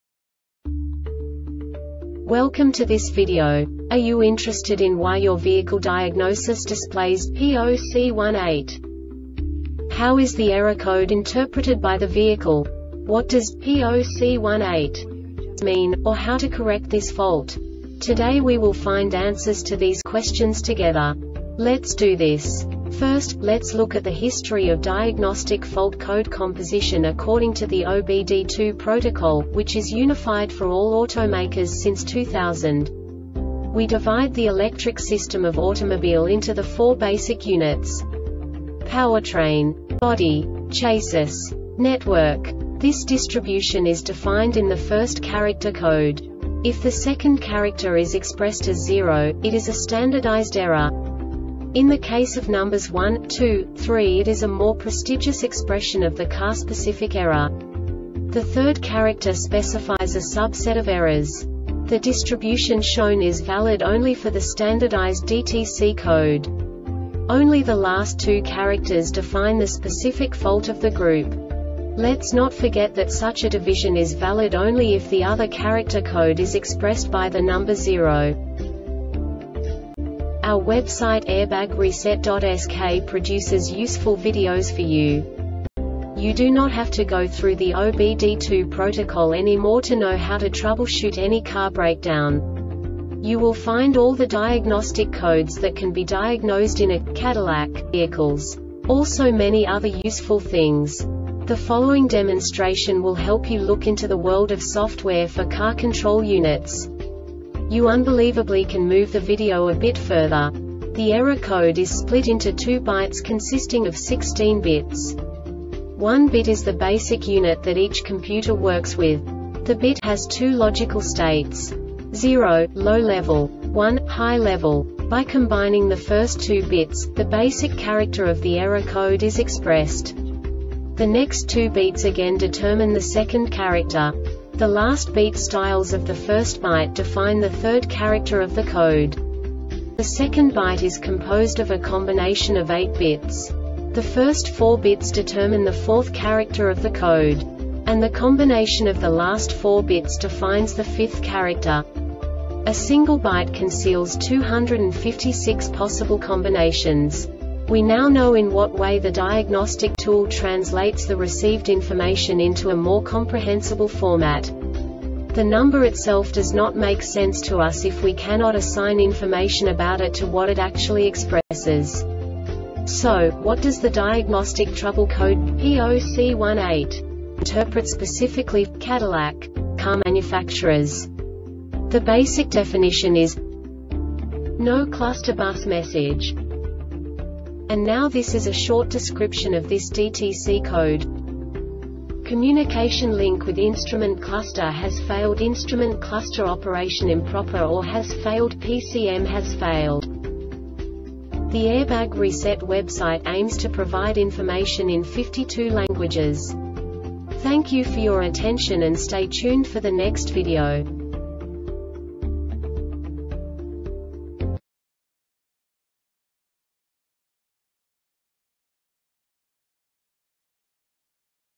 DTC cadillac P0C18 Short Explanation
Contents:
0:21 Basic DTC analysis according to OBD2 protocol standard.
1:48 Insight into programming
2:58 A diagnostically understandable description of the Diagnostic Trouble Code
3:05 Basic error definition

Make:
Cadillac
Code:
P0C18
Definition:
No Cluster Bus Message
Cause: